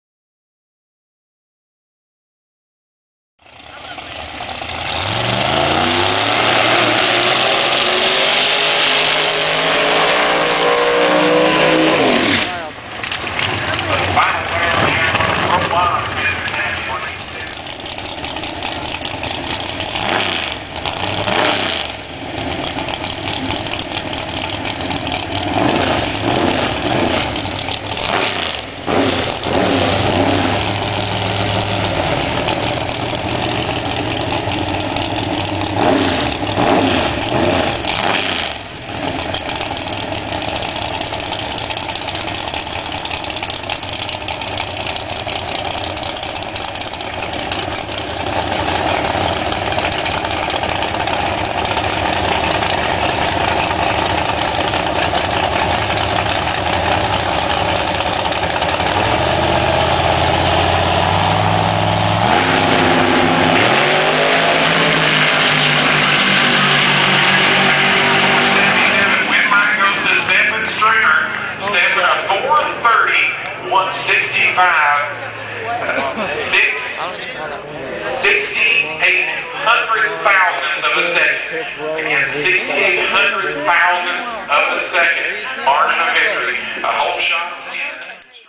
Corvette & Camaro ProMods @ London Dragway 08/29/2009 (Finals)
Camaro + Corvette ProMods race the 1/8th mile of London Dragway, London KY on 08/29/09. Final round of Eliminations.